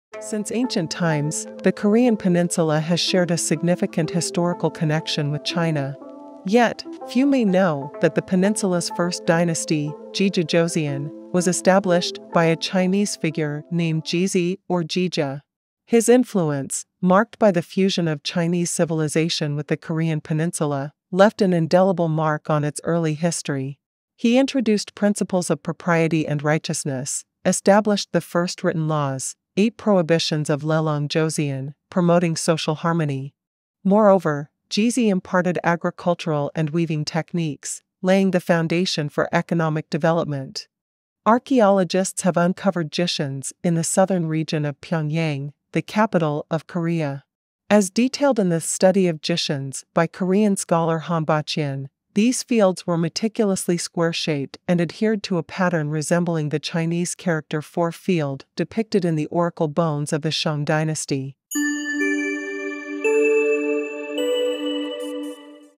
GanJingWorld Exclusive Content | Gija Joseon, the ancient Korean kingdom founded by Jizi or Gija
GanJingWorld Exclusive Content

Gija Joseon, the ancient Korean kingdom founded by Jizi or Gija | Sara’s History of China